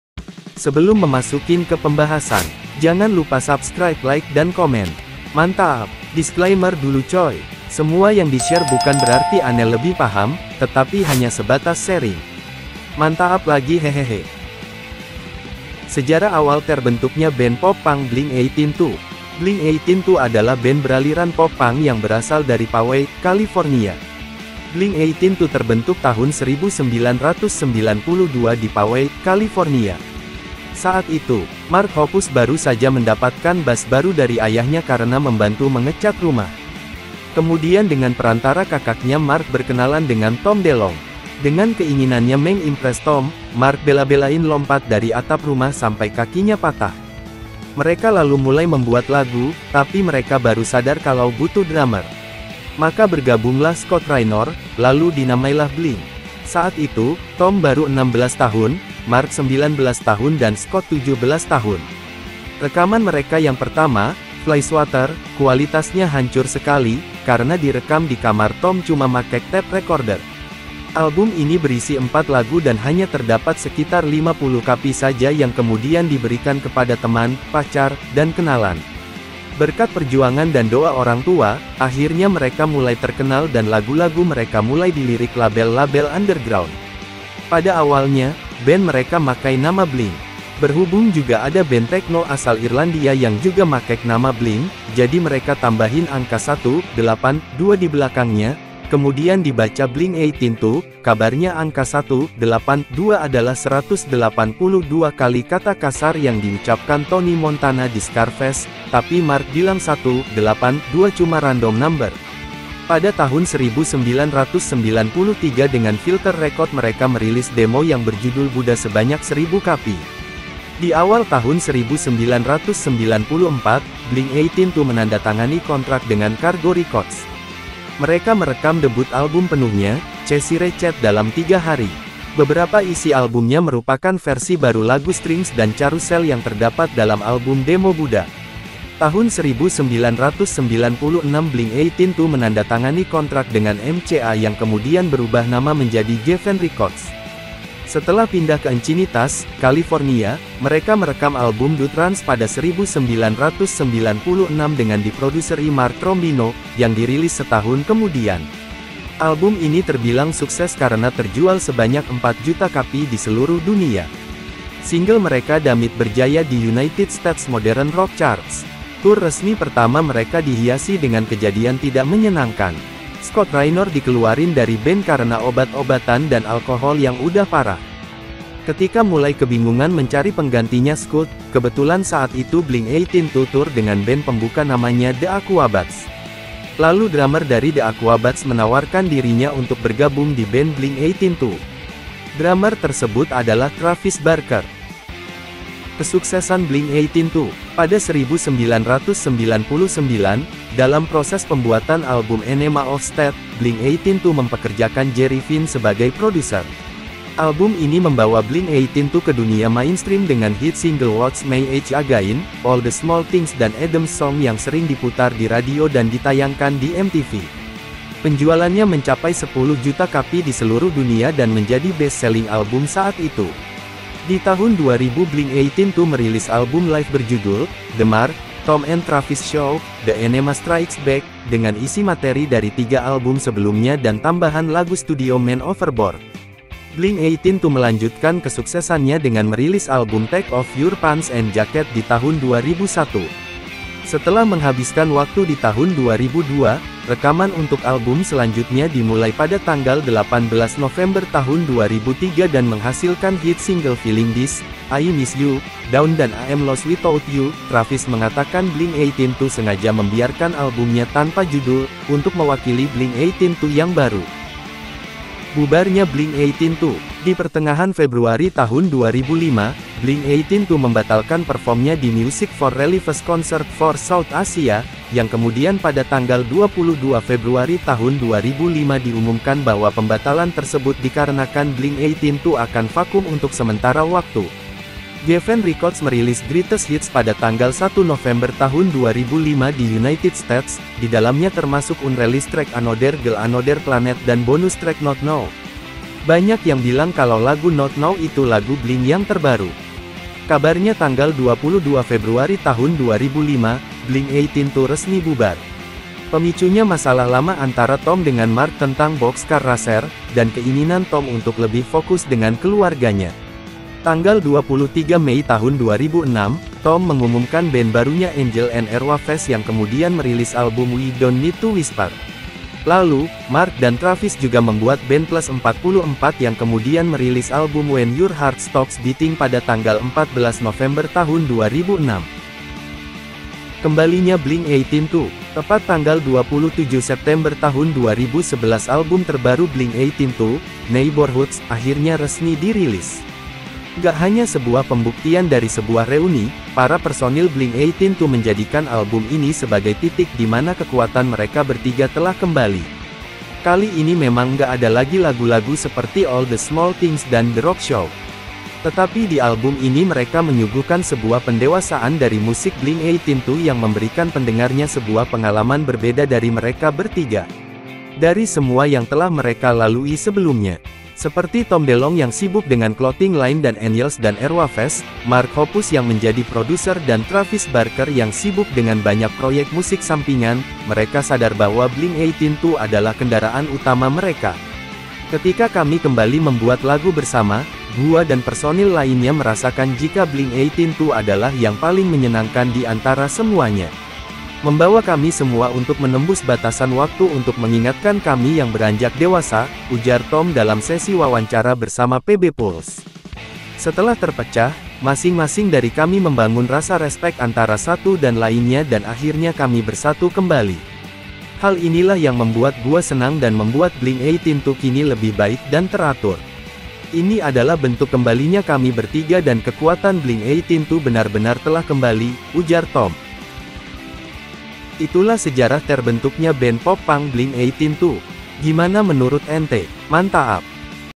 BLINK 182 : BAND POP PUNK DUNIA YANG PENUH LIKA LIKU HINGGA SEMPAT BUBAR
Terima kasih
salam HeadBang!